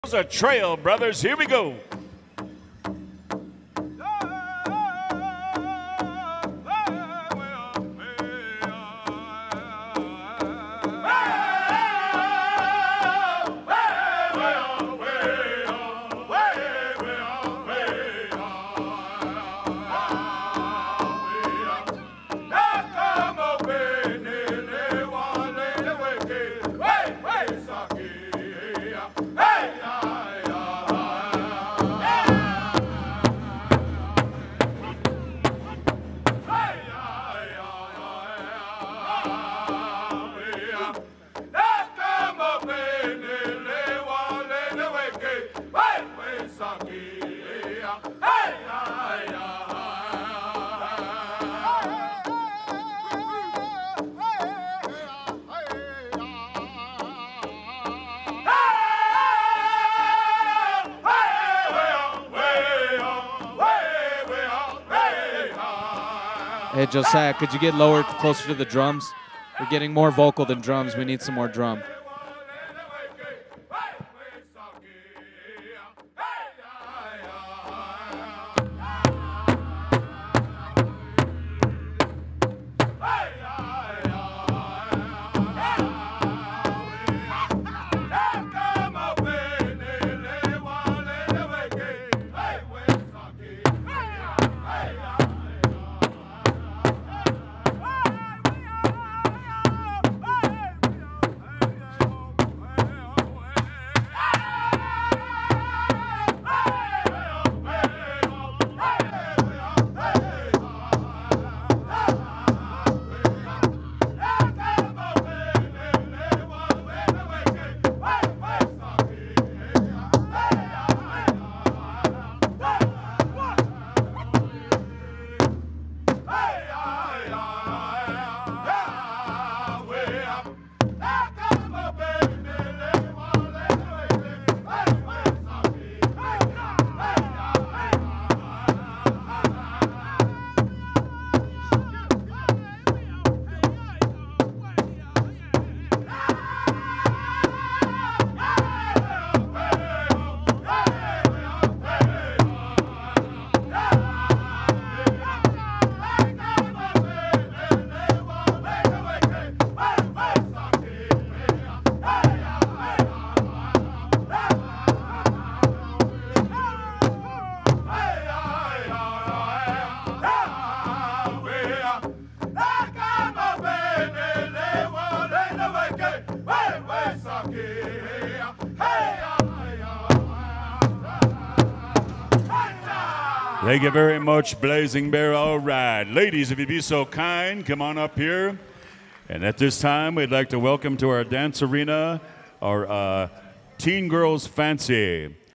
Jr Girls Traditional - 2024 Gathering of Nations Pow Wow
Jr Girls Traditional
Gathering of Nations Pow Wow

Powwow Grounds Expo New Mexico/Tingley Coliseum
Albuquerque, NM